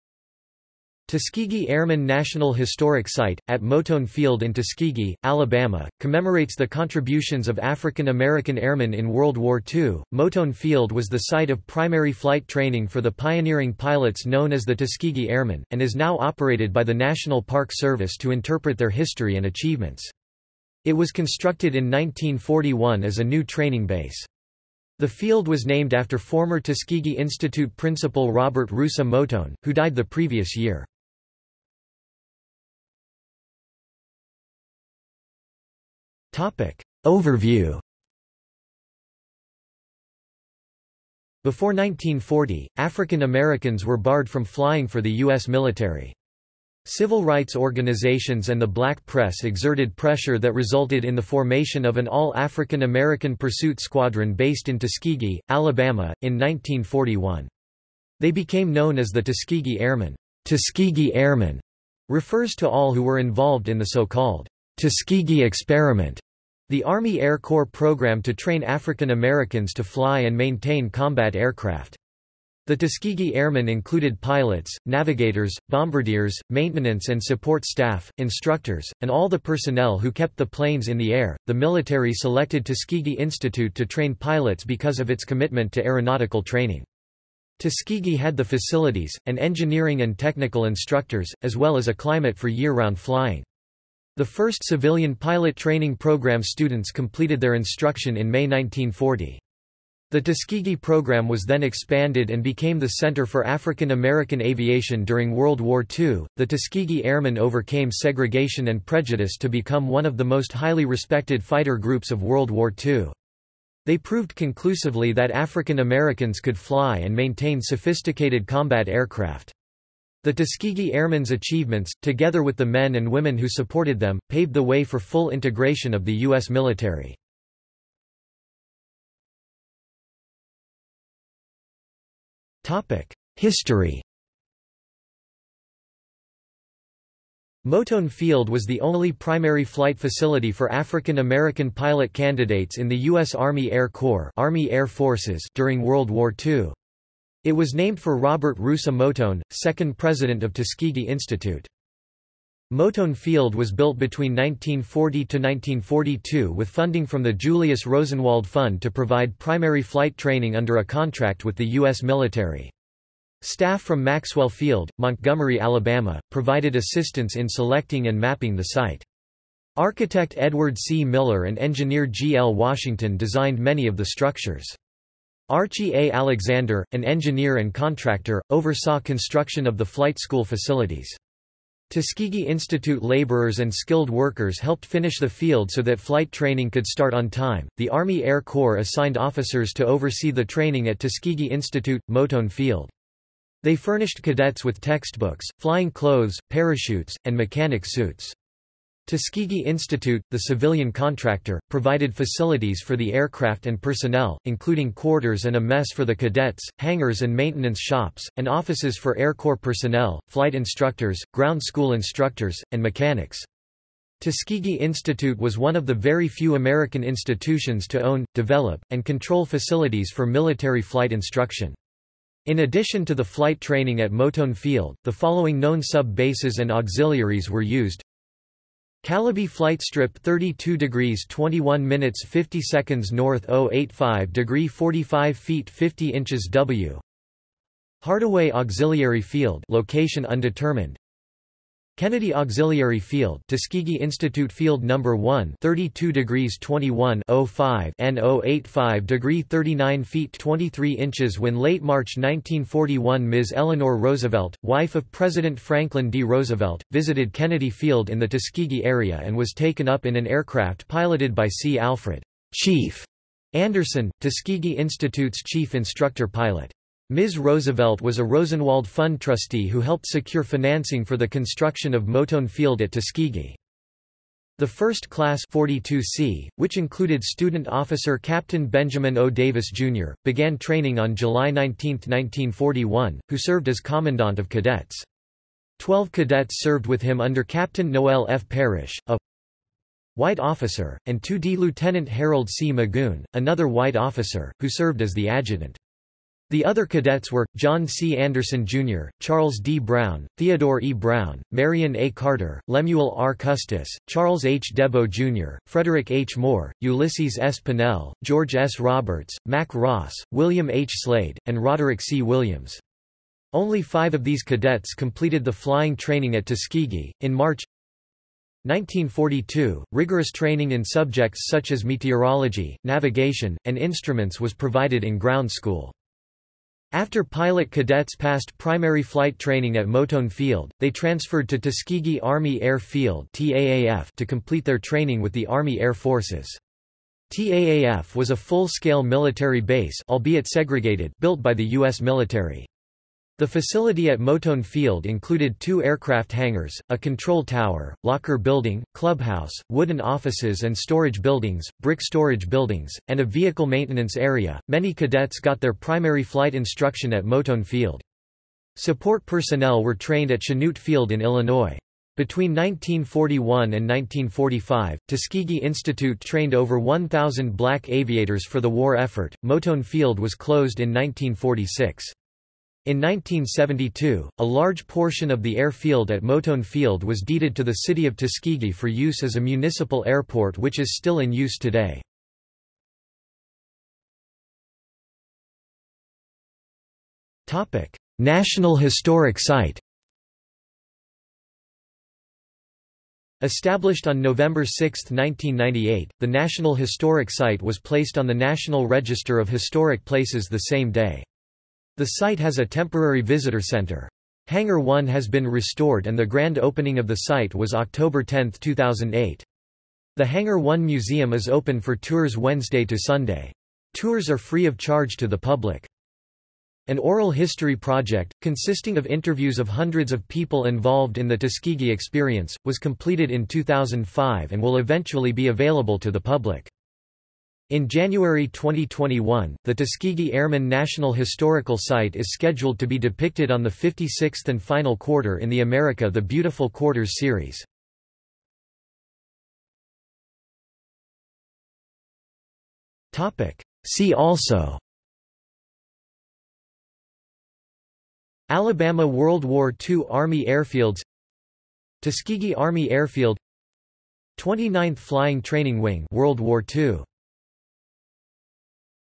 Tuskegee Airmen National Historic Site | Wikipedia audio article
This is an audio version of the Wikipedia Article:
Tuskegee Airmen National Historic Site

00:00:32 1 Overview
00:02:09 2 History
00:06:23 3 National Historic Site
00:07:19 4 See also

- Socrates

SUMMARY
Tuskegee Airmen National Historic Site, at Moton Field in Tuskegee, Alabama, commemorates the contributions of African American airmen in World War II.  Moton Field was the site of primary flight training for the pioneering pilots known as the Tuskegee Airmen, and is now operated by the National Park Service to interpret their history and achievements.  It was constructed in 1941 as a new training base. The field was named after former Tuskegee Institute principal Robert Russa Moton, who died the previous year.